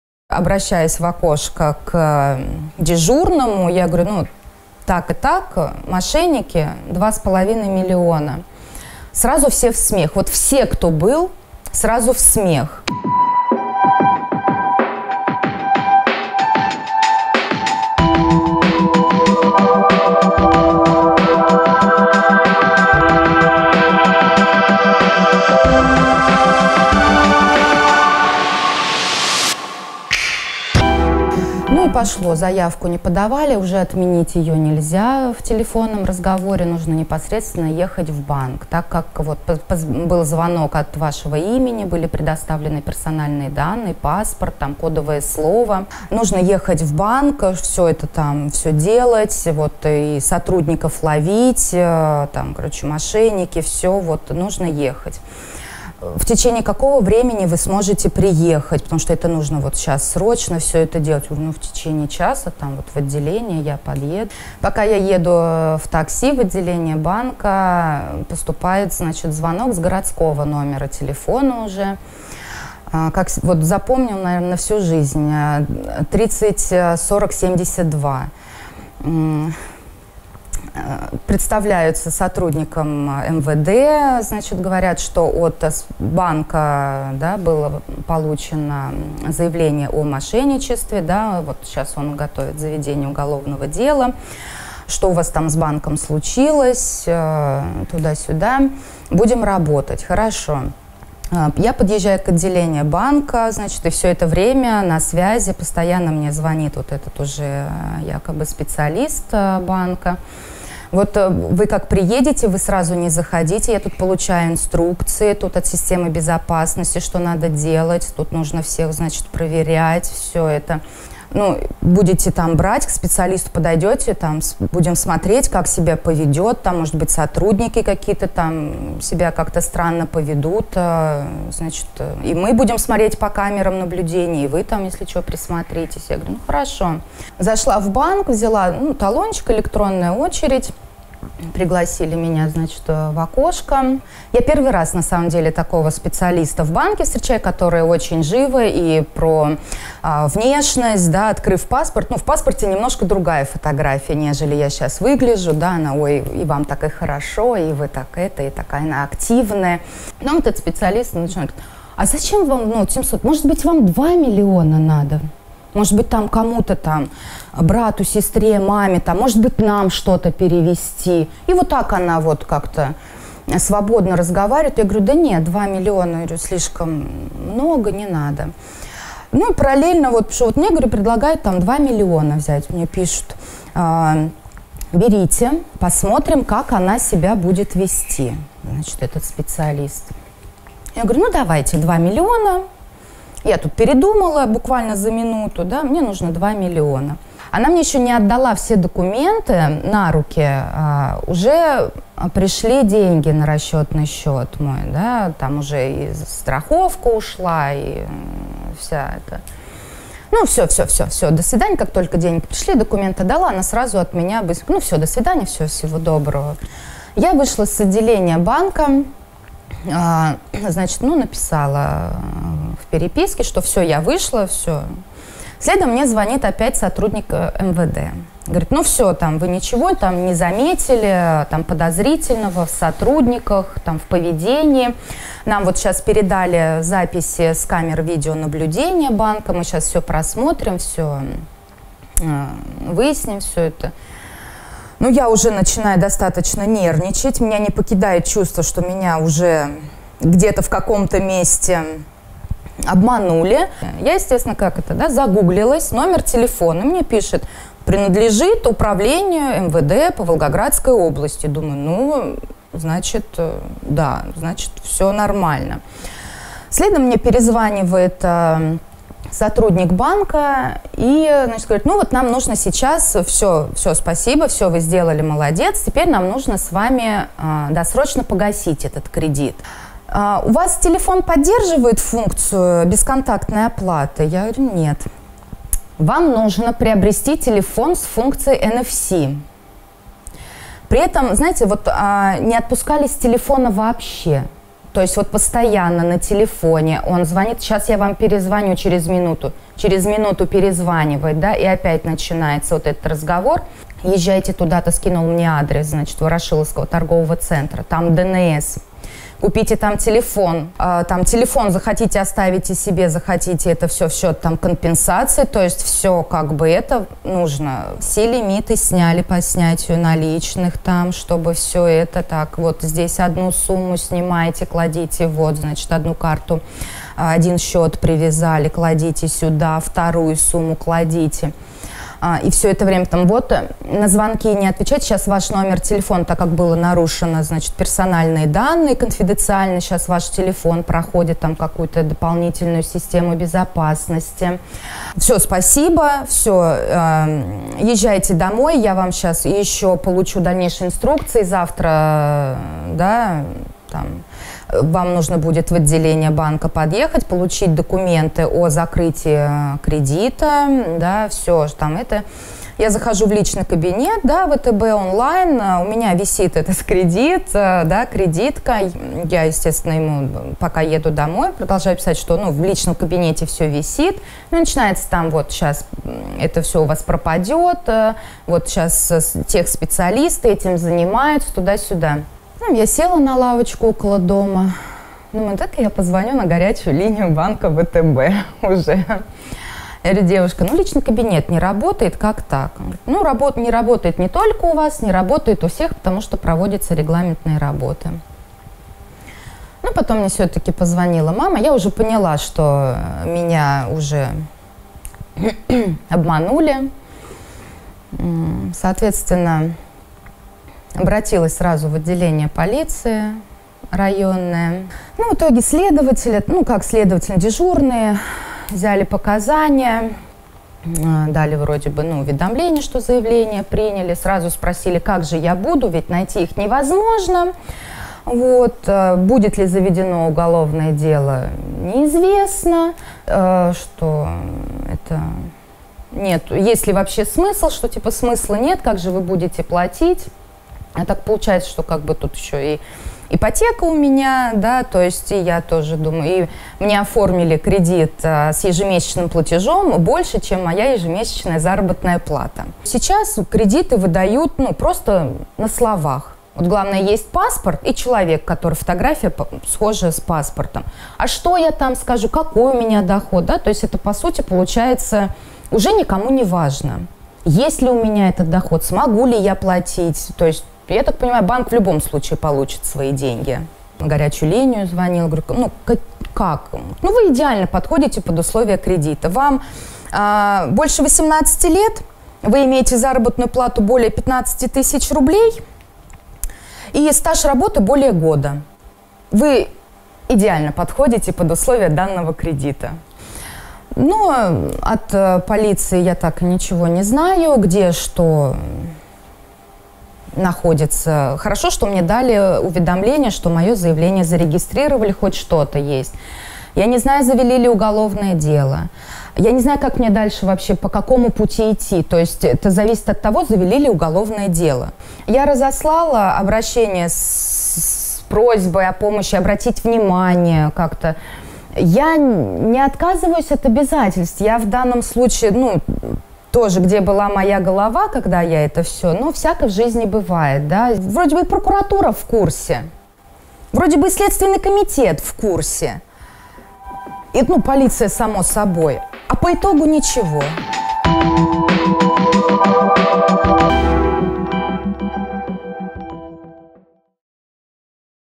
Волгоградка отдала больше миллиона телефонным мошенникам| V1.RU
Настойчивые телефонные звонки от мошенников стали в последнее время настоящим бедствием для тысяч и тысяч россиян. Казалось, что все знают — если вам звонят из банка и сообщают о попытке взлома вашего личного кабинета, попытке взять кредит или требуют перевести деньги на безопасный счет, самым лучшим случаем спасти свои финансы станет окончание разговора. Но, даже те, кто знает об опасности подобных звонков, часто не выдерживают морального прессинга и лишаются сбережений или загоняют себя в огромные долги. Так, волгоградка Светлана отдала мошенникам больше двух миллионов рублей и теперь не знает, как жить дальше с огромным долгом. Она согласилась рассказать свою историю, чтобы уберечь других.